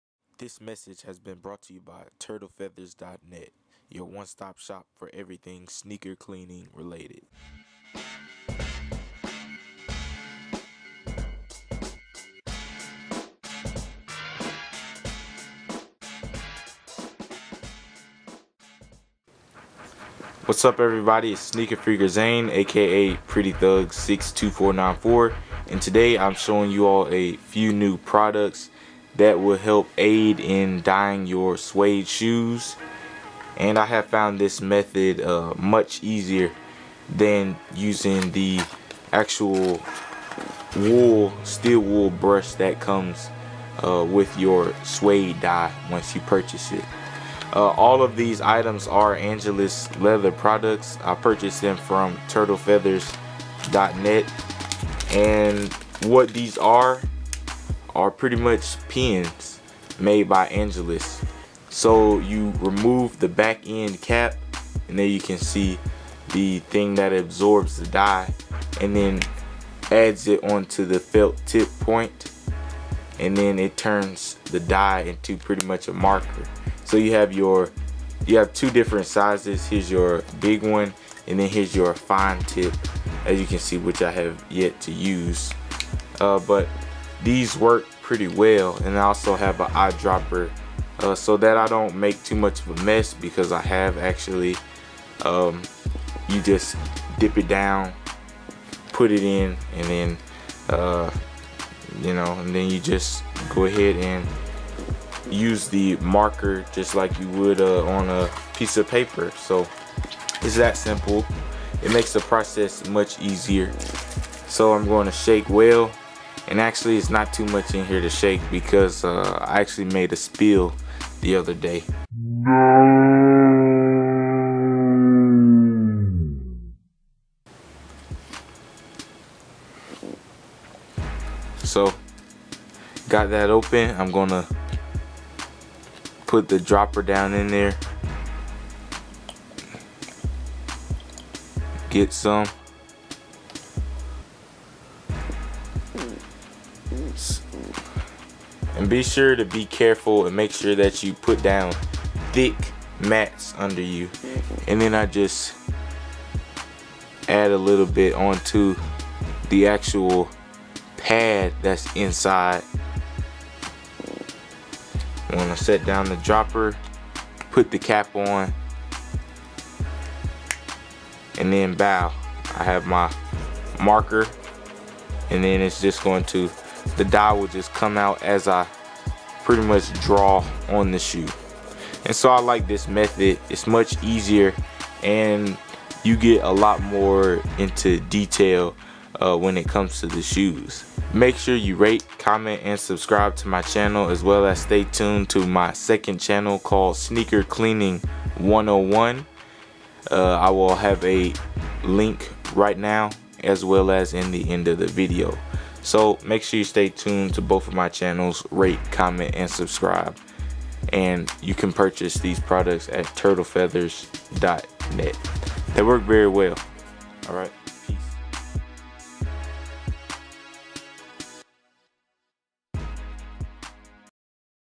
Dying Suede Shoes Tutorial Pt. 2
Supplies:
- Angelus Suede/Nubuck Dye (Color of your Choice)
- Rags
- Tape (Masking, Painters, etc.)
- Cutting Utencil (Box Cutter, Razor, etc.)

Instructions:

Step 1:
Tape up areas of the shoe that are not desired to be dyed.

Step 2:
Cut off surrounding edges of the tape to have a defined shape cutout.

Step 3:
Dip the brush into the Angelus Dye and brush across the suede.nubuck surface.

Step 4:
Color up desired area completely and then let sit.

Step 5:
Clean up